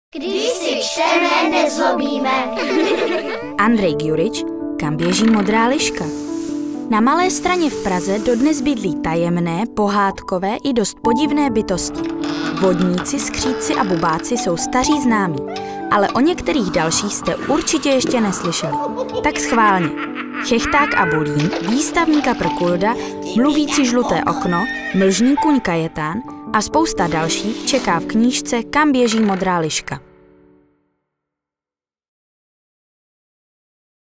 Gjuric, A: Kam běží modrá liška
Kam běží modrá liška? - Na Kampu přece. Nové vydání legendární dětské knihy Andreje Gjuriče, která v jedinečném rytmu vypráví příběhy z Malé Strany, vychází více než po třiceti letech od prvního vydání. Gjurič v ní vytvořil vlastní mytologii, svět zabydlený bubáky, vodníky, ale i obyčejnými lidmi. Jedno z nejmagičtějších míst Prahy, tak ožívá v novodobých mýtech, které svou působivostí okouzlí nejen děti, ale i rodiče, kteří jim příběhy budou číst.